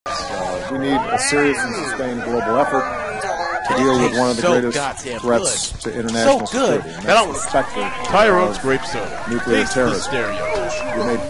Is this racist Obama?
President Obama gives a speech on chewing watermelon, drinking grape soda and eating fried chicken.